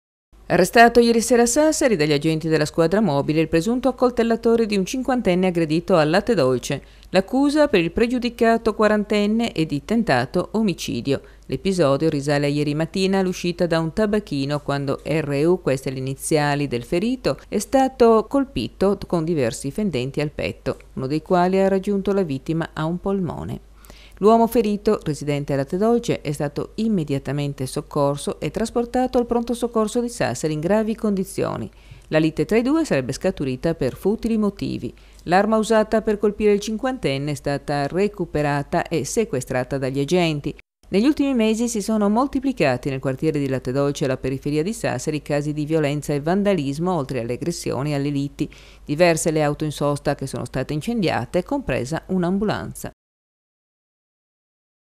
SASSARI, ARRESTATO IL PRESUNTO ACCOLTELLATORE DI LATTE DOLCE
Arrestato ieri sera a Sassari, dagli agenti della squadra mobile, il presunto accoltellatore di un cinquantenne aggredito a Latte Dolce. L’accusa per il pregiudicato quarantenne è di tentato omicidio